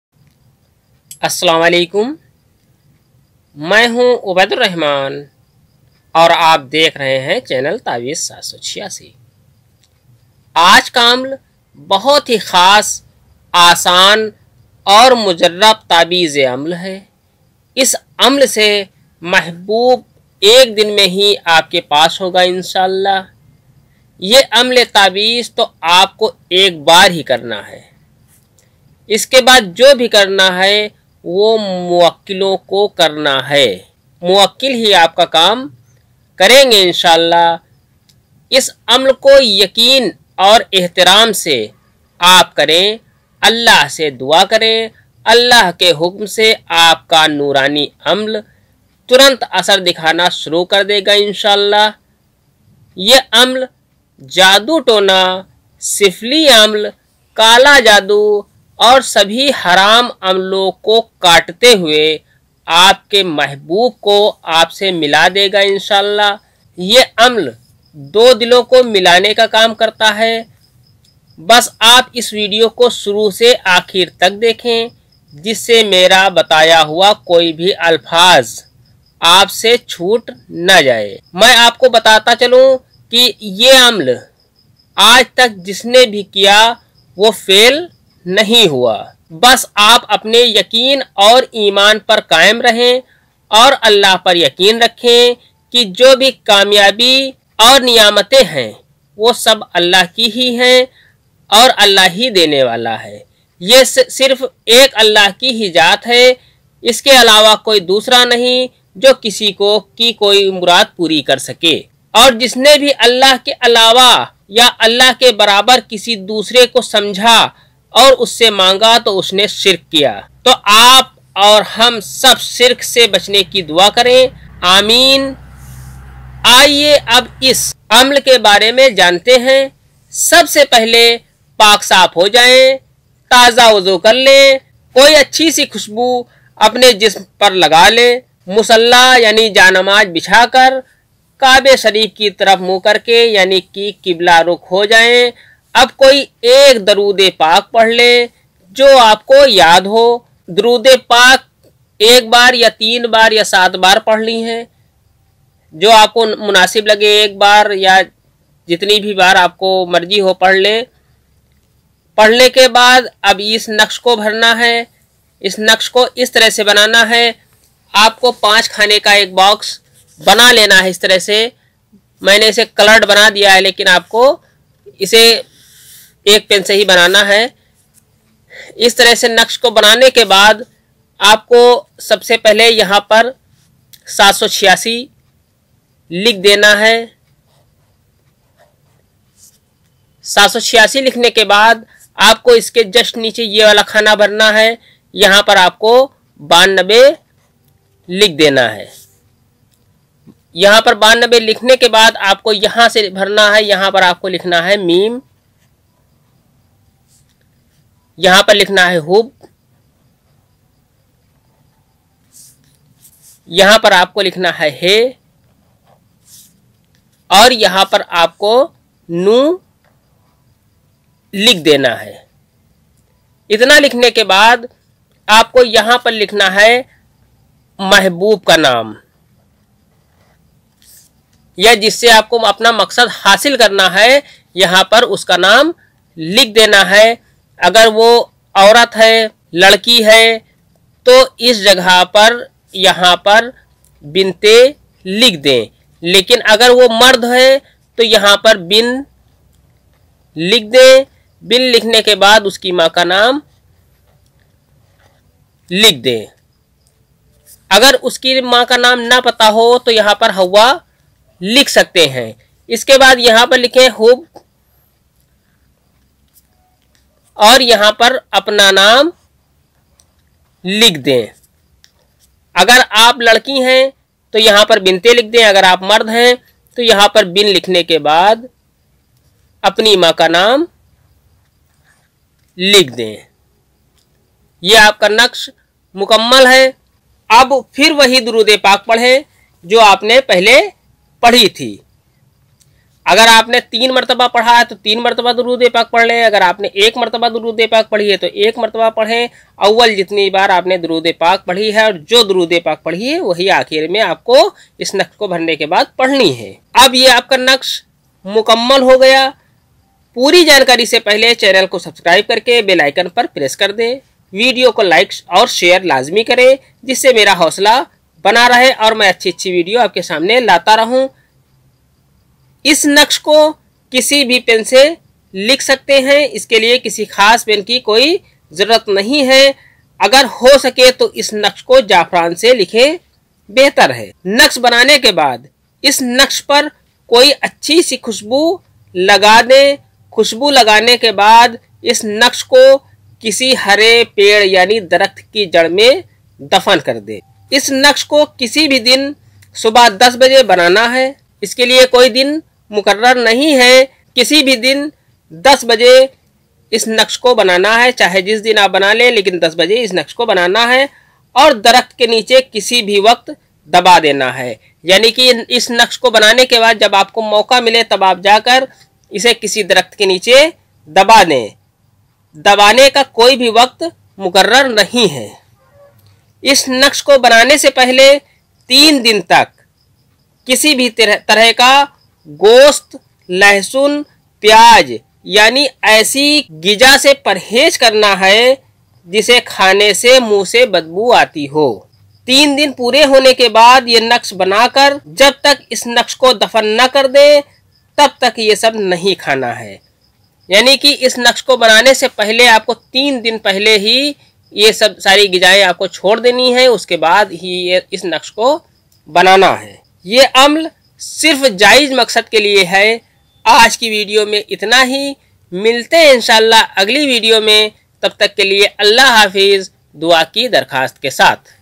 Ek Din Me Hi Mahboob Ko Apne Bas Me Karne Ka Taweez - Kisi Ko Bhi Bas Me Kar Ke Gulam Bana De Naqsh
ek bar ke istemal se hi kisi ko bhi bas me kar dene wala aur gulam bana dene wala taweez

ya amal mohabbat aur bas me karne ka bahut motbar naqsh e azeem hai is taweez ke istemal se aap kisi ko bhi bas me kar sakte hai

Naqsh E Aamleen E Hub Mohabbat Me Gulam Bana Dene Wala Taweez Muwakkil Khud Mohabbat Se Milayenge